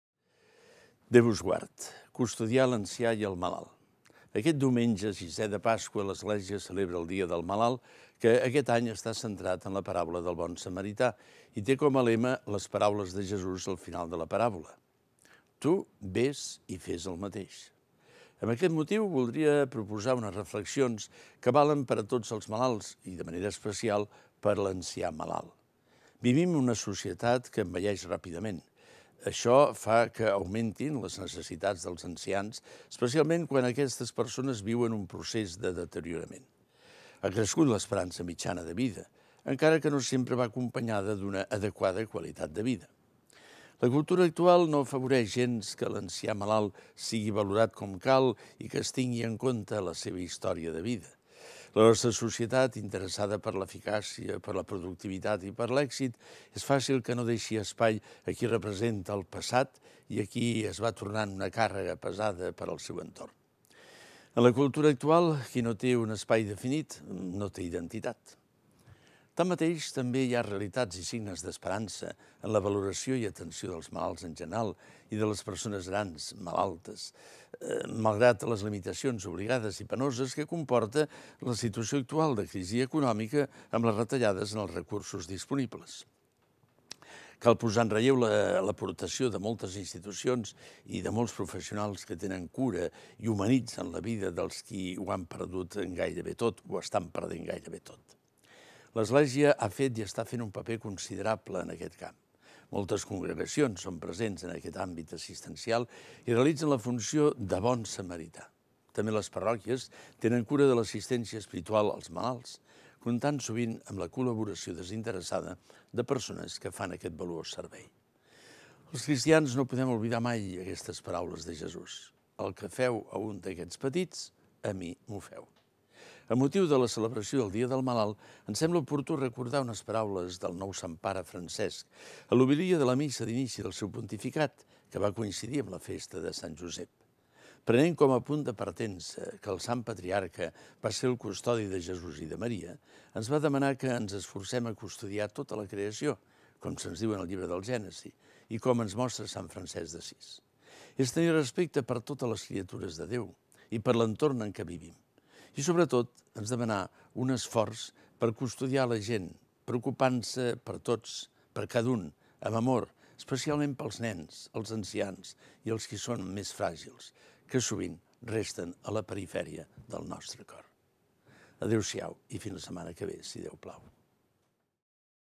PARAULA I VIDA: Custodiar l'ancià i el malalt
PARAULA I VIDA (Diumenge, 5/05/2013)

Aquest diumenge sisè de Pasqua l'Església celebra el Dia del Malalt, que aquest any està centrat en la paràbola del bon samarità i té com a lema les paraules de Jesús al final de la paràbola: "Tu, vés i fes el mateix" (Lc 10,37). Amb aquest motiu voldria proposar unes reflexions que valen per a tots els malalts i de manera especial per a l'ancià malalt.

Vivim en una societat que envelleix ràpidament. Això fa que augmentin les necessitats dels ancians, especialment quan aquestes persones viuen un procés de deteriorament. Ha crescut l'esperança mitjana de vida, encara que no sempre va acompanyada d'una adequada qualitat de vida.

La cultura actual no afavoreix gens que l'ancià malalt sigui valorat com cal i que es tingui en compte la seva història de vida. La nostra societat, interessada per l'eficàcia, per la productivitat i per l'èxit, és fàcil que no deixi espai a qui representa el passat i es va tornant una càrrega pesada per al seu entorn. En la cultura actual, qui no té un espai definit no té identitat.

Tanmateix també hi ha realitats i signes d'esperança en la valoració i atenció als malalts en general i a les persones grans malalts, malgrat les limitacions obligades i penoses que comporta la situació actual de crisi econòmica, amb les retallades en els recursos disponibles. Cal posar en relleu l'aportació de moltes institucions i de molts professionals que tenen cura i humanitzen la vida dels qui ho van perdent gairebé tot.

L'Església ha fet i està fent un paper considerable en aquest camp. Moltes congregacions són presents en aquest àmbit assistencial i realitzen la funció del bon samarità. També les parròquies tenen cura de l'assistència espiritual als malalts, comptant sovint amb la col•laboració desinteressada de persones que fan aquest valuós servei.

Els cristians no podem oblidar mai aquestes paraules de Jesús: "El que feu a un d'aquests petits, a mi m'ho feu". Amb motiu de la celebració del Dia del Malalt, em sembla oportú recordar unes paraules del nou Sant Pare Francesc en l'homilia de la missa d'inici del seu pontificat, que va coincidir amb la festa de Sant Josep. Prenent com a punt de partença que el sant patriarca va ser el custodi de Jesús i de Maria, ens va demanar que ens esforcem a custodiar tota la creació, com se'ns diu en el llibre del Gènesi i com ens mostra sant Francesc d'Assís: és tenir respecte per totes les criatures de Déu i per l'entorn en què vivim. I sobretot ens demanà un esforç "per custodiar la gent, preocupar-se per tots, per cada un, amb amor, especialment pels nens, els ancians i els qui són més fràgils, que sovint resten a la perifèria del nostre cor".

+ Lluís Martínez Sistach
Cardenal arquebisbe de Barcelona